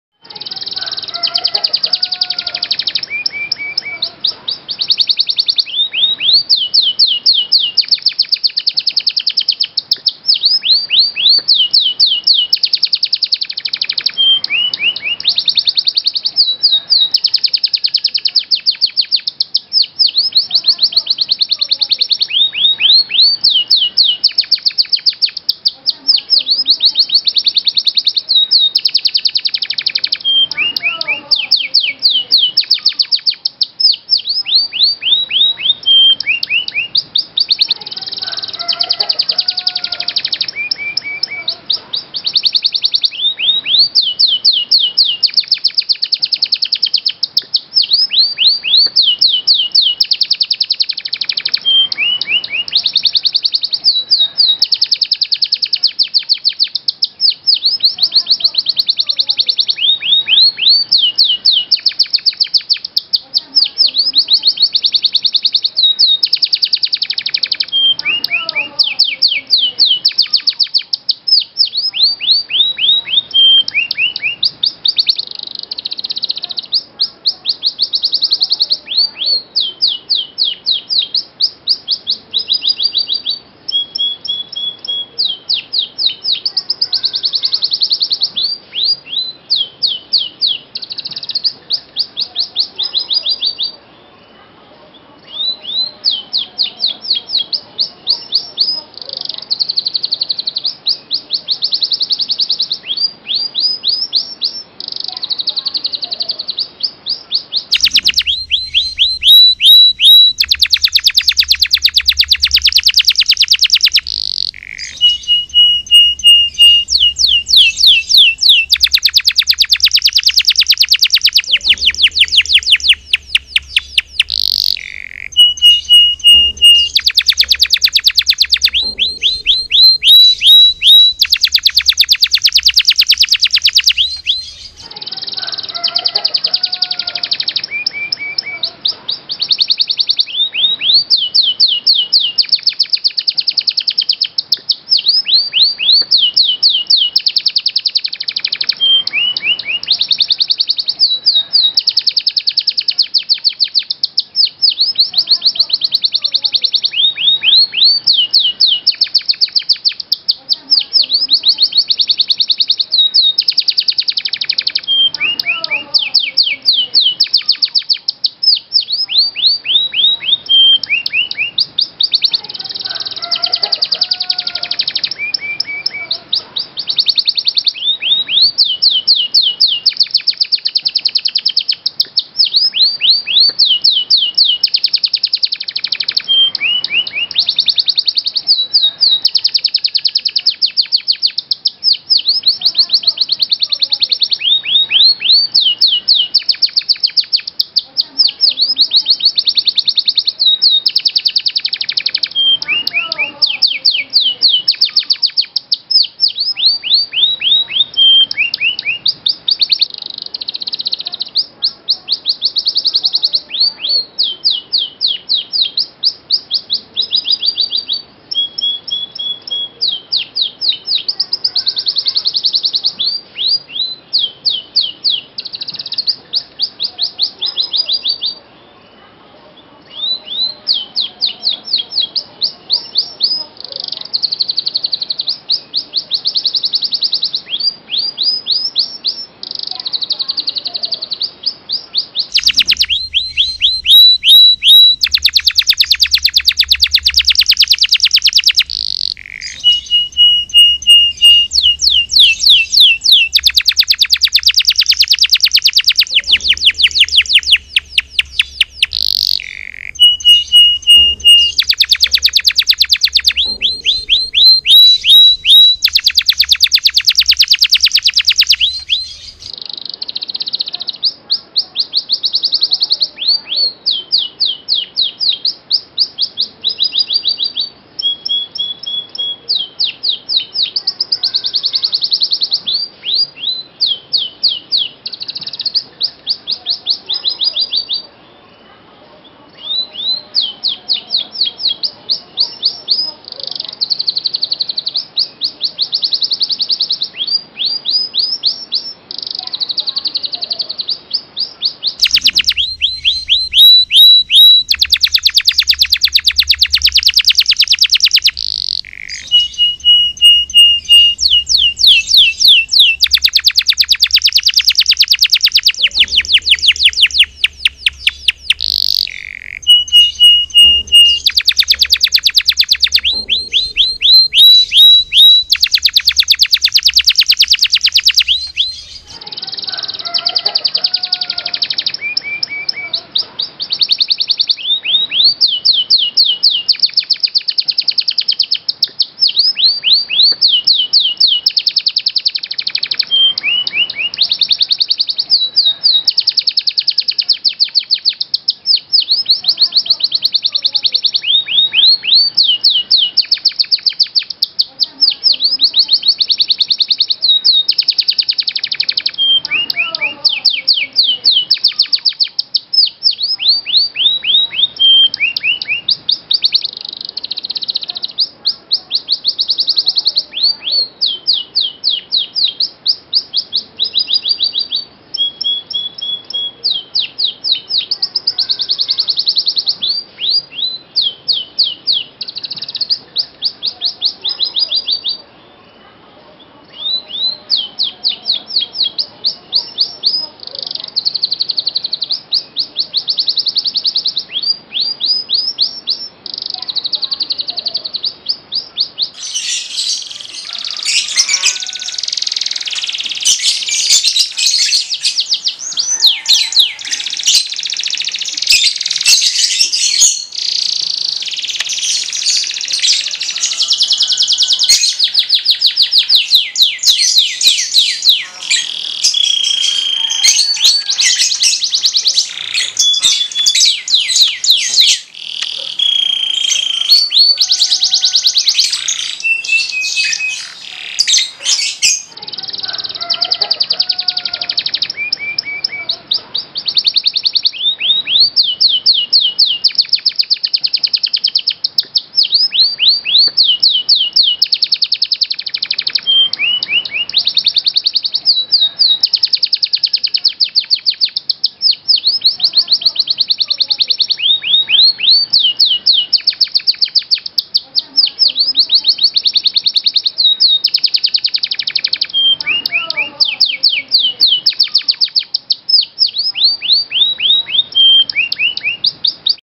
pancingan kenari males bunyi
Terapi suara burung kenari buat burung krnari males bunyi